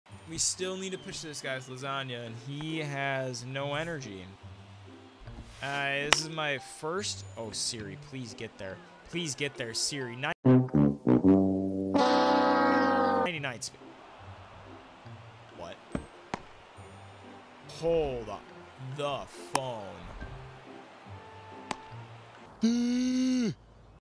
I MADE AN OOPSY POOPSY WITH SUPERCHARGED JOSE SIRI
Jose Siri was a 99 supercharged CF, but I may have done him a bit dirty on this play